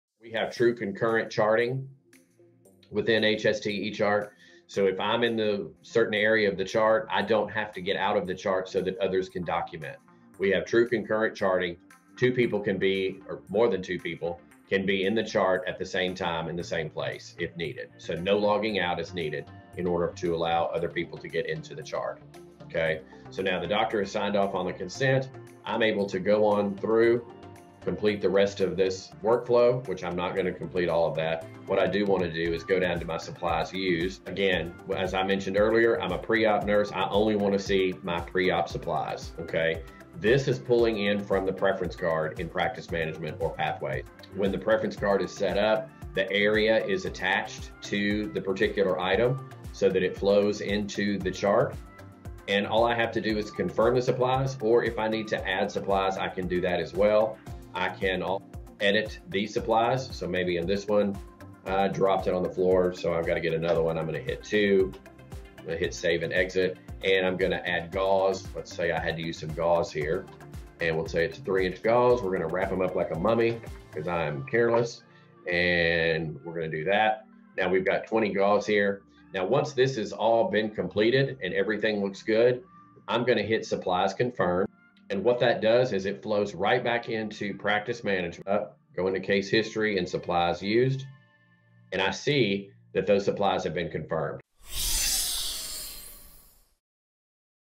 Navigating Supply Lists with Precision: HST's Electronic Charting in Action
Delve into the intricate functionalities of HST's electronic charting, focusing on its seamless supply list capabilities. Learn how to swiftly navigate through pre-op supplies, integrate with HST's surgery center management, and ensure accurate data logging with HST's electronic charting. From the doctor's consent to tracking supply usage, watch how the entire process is streamlined for maximum efficiency.